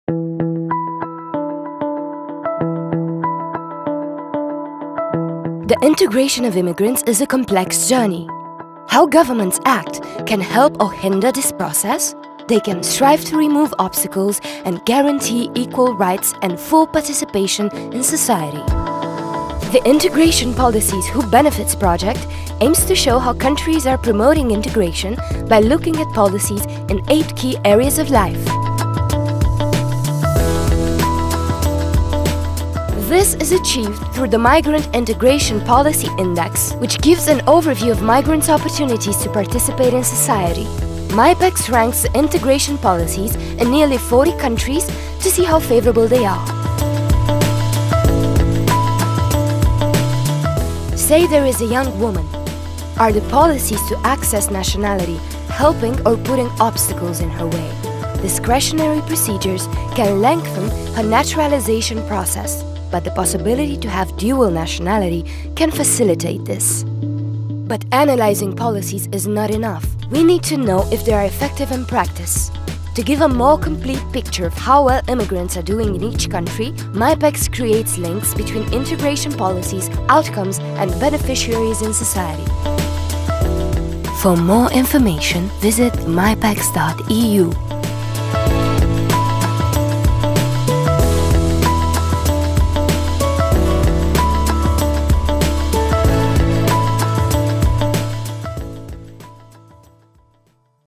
What is MIPEX?
This video presents the "INTEGRATION POLICIES: WHO BENEFITS?" project and its main outcome - MIPEX (Migrant Integration Policy Index).

The Migrant Integration Policy Index (MIPEX) is a unique tool which measures policies to integrate migrants in all EU Member States, Australia, Canada, Iceland, Japan, South Korea, New Zealand, Norway, Switzerland, Turkey and the USA.
167 policy indicators have been developed to create a rich, multi-dimensional picture of migrants’ opportunities to participate in society. The index is a useful tool to evaluate and compare what governments are doing to promote the integration of migrants in all the countries analysed.
The project informs and engages key policy actors about how to use indicators to improve integration governance and policy effectiveness.